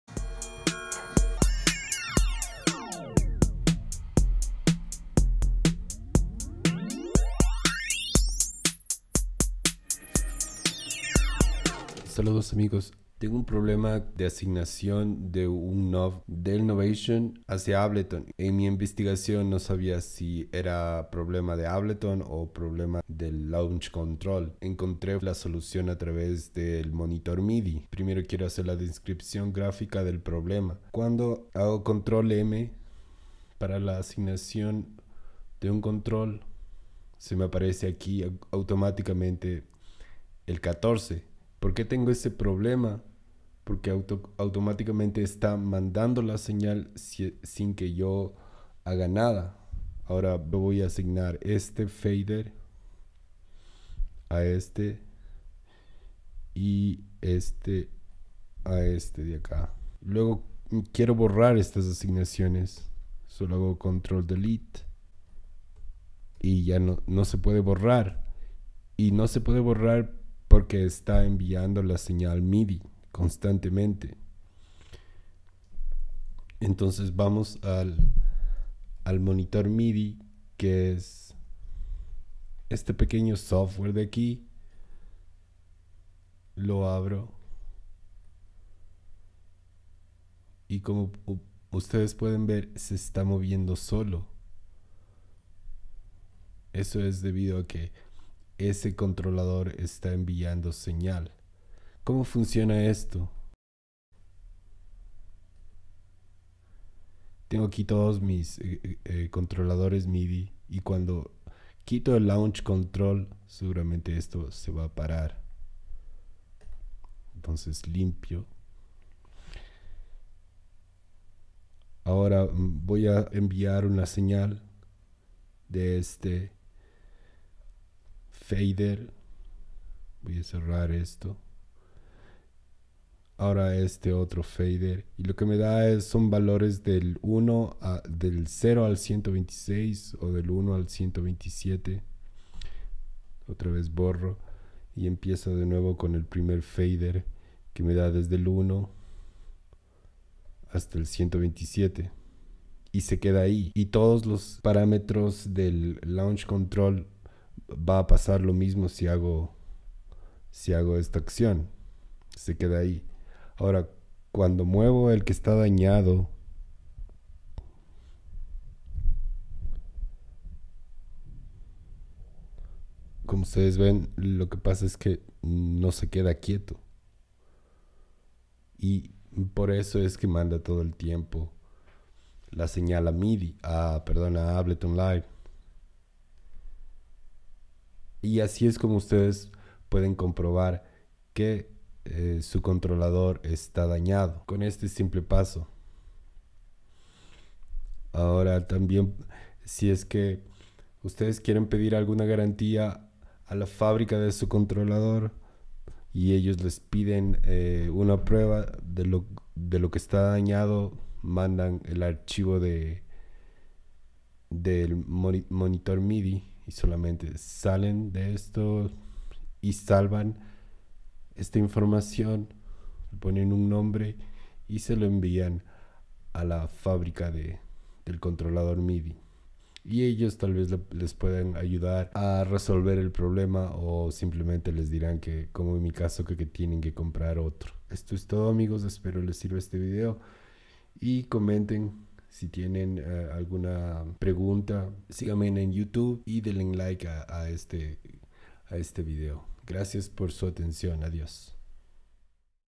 Como funciona el Monitor MIDI, comprueba si tu controlador MIDI esta dañado.
No funciona algún parámetro de tu controlador MIDI? . Con este video puedes verificarlo.
Si te ha gustado este video, por favor considera hacer una donación
Gracias
Marco.